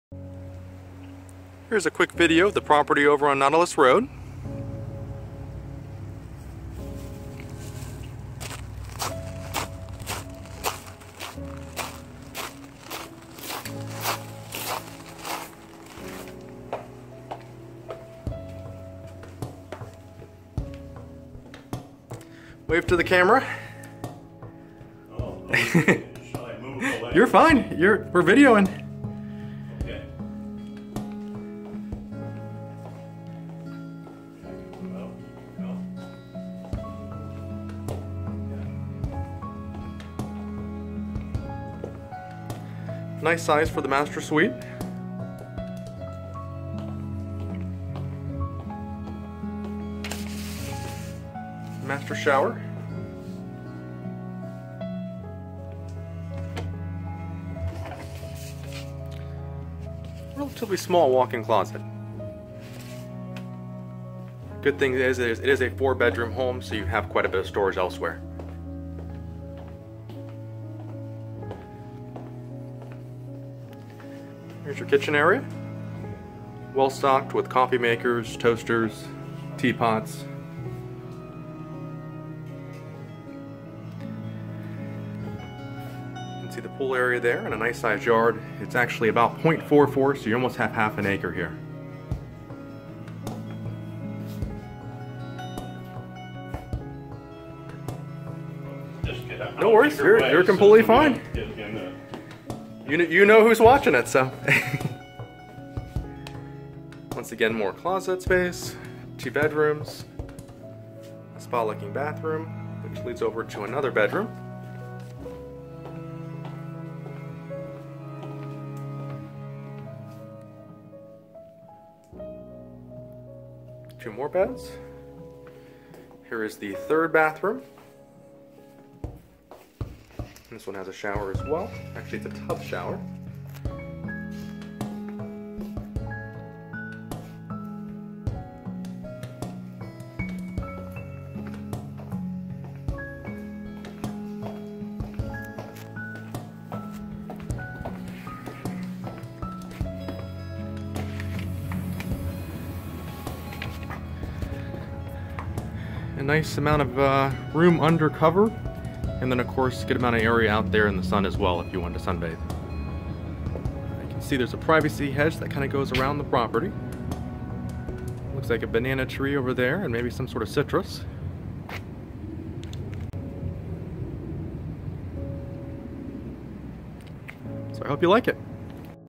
1400 nautilus rd
Owner/Agent. Available June 17, 2017 for Annual lease . Seasonal available for 2018 at this point. Fully furnished 4/3 with screened, heated pool on a large well-landscaped private corner lot. Off season or annual at $4,500./mo. Seasonal leases have a 4 month minimum at $9,000./month. Shoulder months at $7500./month. No showings during tenant occupancies. Floor plan/photo on MLS. Walk to the white sandy beach at Lowdermilk Park with it's restaurant, showers, volleyball courts and play area. The Naples Beach Club has golf, tennis and a spa available. This unique, quiet neighborhood called Coquina Sands is great for long walks or bike rides and is close to beaches, restaurants, shops, the Coastland Mall & the historic section of downtown Naples with Fifth Avenue and Third Street South.The floor plan features a new kitchen with family room & updated baths. The master bedroom suite is separated from the 3 guest bedrooms. Beige tile flooring, crown molding throughout and plantation shutters provide a neutral decor and combine to make this a great vacation house for friends and family ! Just bring your suitcase...everything else is waiting for you. Owner/Agent.